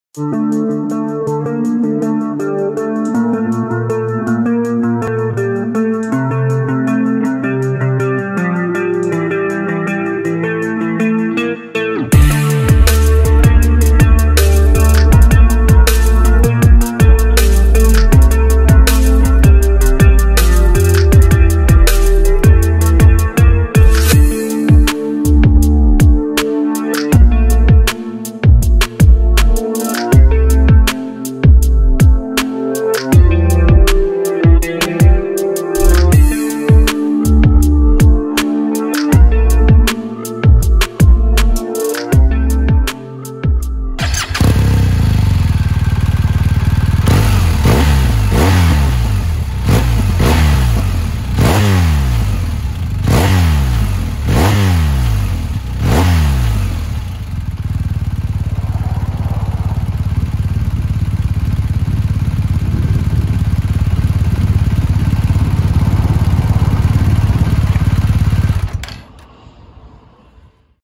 2022 BMW GS1250 TE TRIPLE BLACK - WALKAROUND AND START UP
Short walkaround and start up of a stock 2022 BMW GS1250 TE Triple Black.

Catch you on the next one!